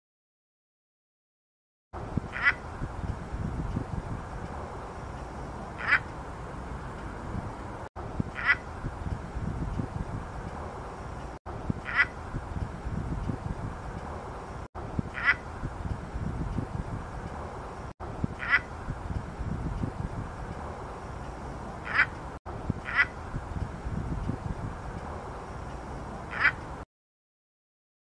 Crested ibis
This large, white ibis is native to East Asia and is listed as vulnerable. There are only about 10,000 individuals left in the wild. @rinosgarden02